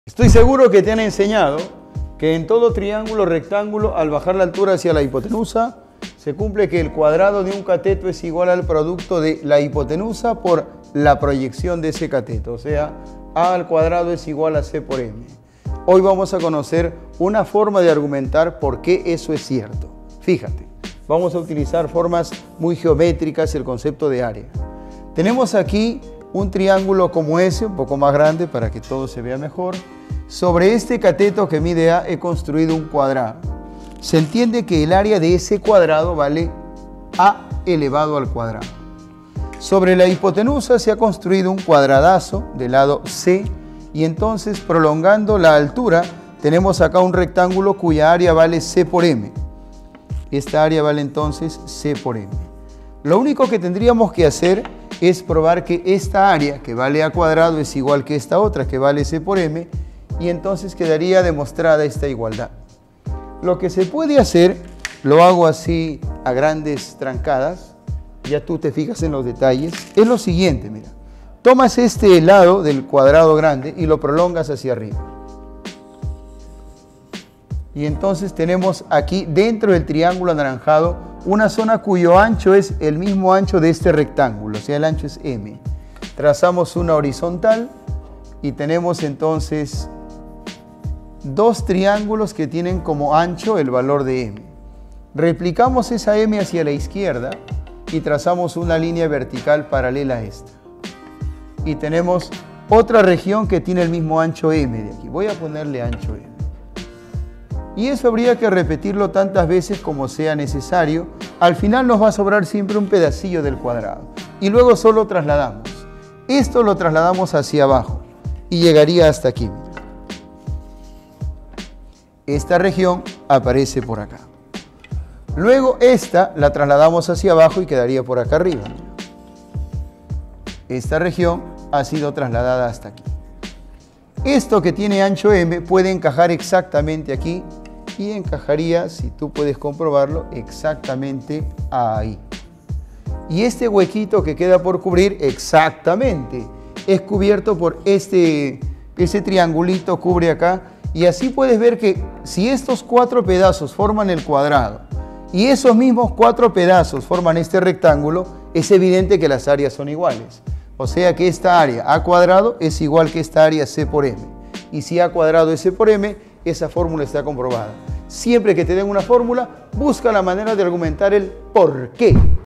¿POR QUÉ? 🤔
El maestro Moisés Rayme nos explica otra propiedad 🧐
Esta vez de un triángulo rectángulo 🤩
📂Inicio de Ciclos📂
🥳 Ciclos presenciales UNMSM el día 31 de Marzo.
🥳Inicio de ciclo virtuales: 24 de Marzo
No esperes más ¡Únete a la familia Vonex!
Tú serás el próximo cachimbo UNMSM